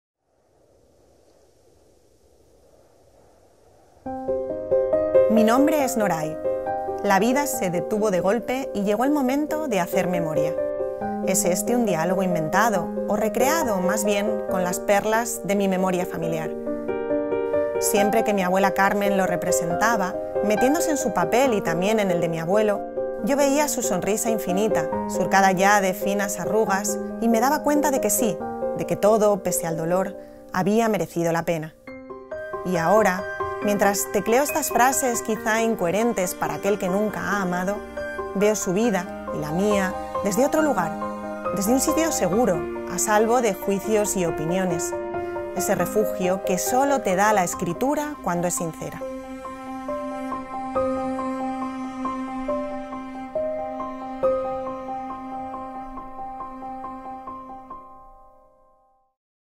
Las formas del querer de Inés Martín Rodrigo - Premio Nadal de Novela 2022 | Ediciones Destino
Inés Martín Rodrigo presenta “Las formas del querer”, la novela ganadora del Premio Nadal 2022. Noray, la protagonista de esta historia, descubrirá las infinitas maneras que tiene el querer a través de los recuerdos de su familia. Cuando la vida se detiene, es momento de saber quiénes somos.